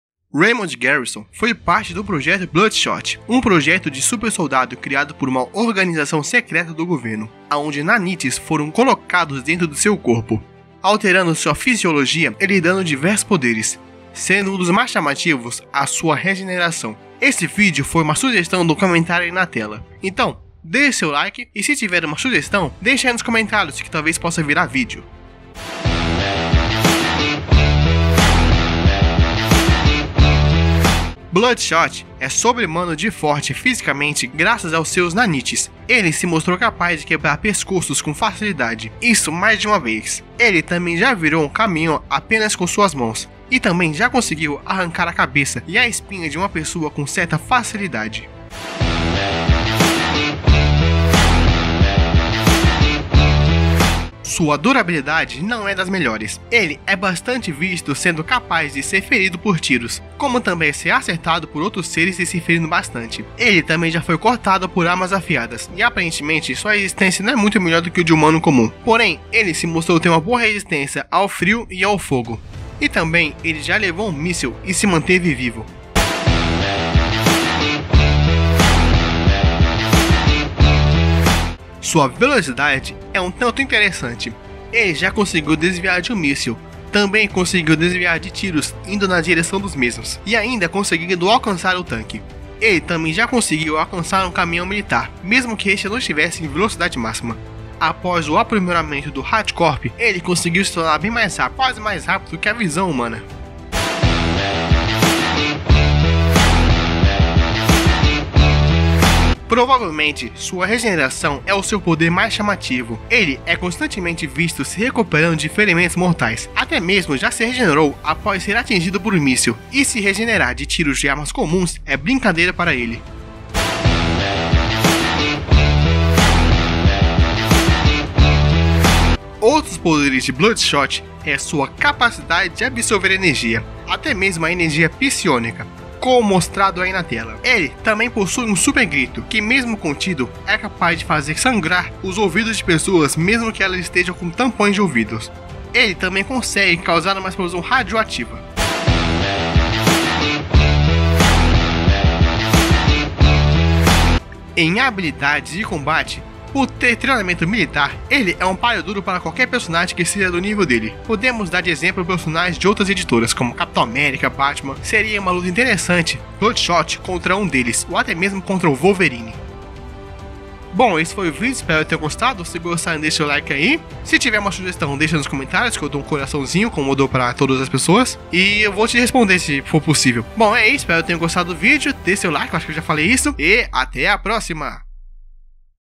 OS PODERES DO SUPER SOLDADO BLOODSHOT | Valiant Comics
Raymond Garrison, foi parte do Projeto Bloodshot, um projeto de super-soldado criado por uma organização secreta do governo, aonde nanites foram colocados dentro de seu corpo, alterando sua fisiologia e lhe dando diversos poderes, sendo um dos mais chamativos a sua regeneração, esse vídeo foi uma sugestão, então deixa seu like, e se tiver uma sugestão, deixa ai nos comentários que talvez possa virar vídeo!

😈 NÃO CLIQUE:
📺 Conteúdos do Canal, clique no link encurtado para ser direcionado a paginar de playlist do meu canal